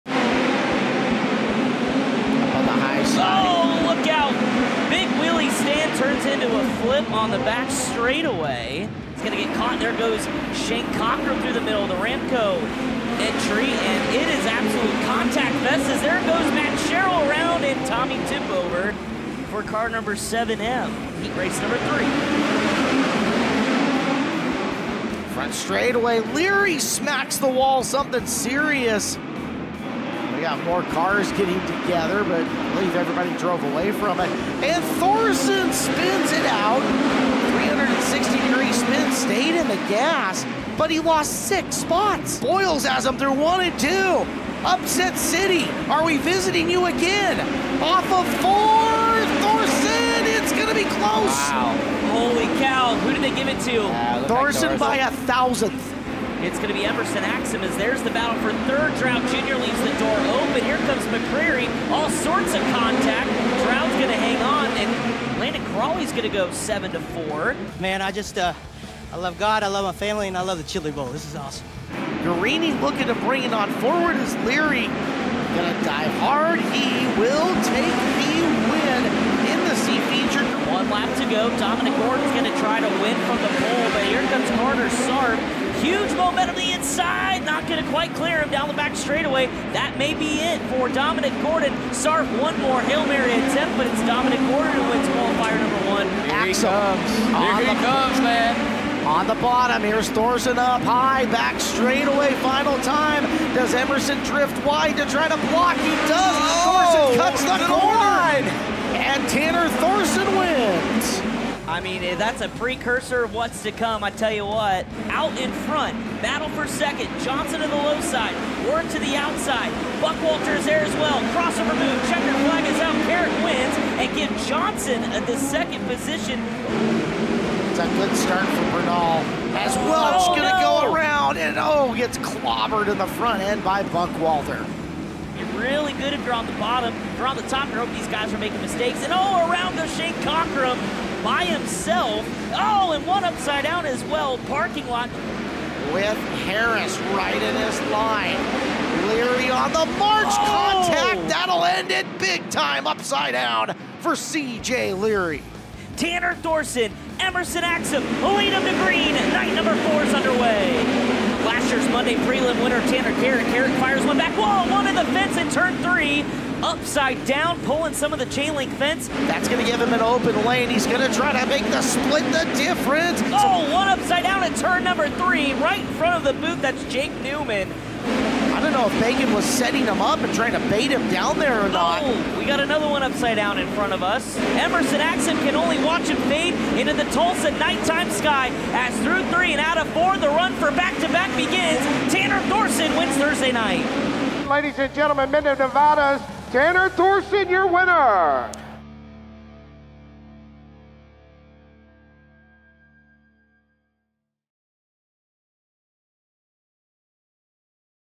Thursday Thrills & Spills | 2023 Lucas Oil Chili Bowl Nationals
Last year's Lucas Oil Chili Bowl champion Tanner Thorson began his title defense in a remarkable way on Thursday night at the Tulsa Expo Center.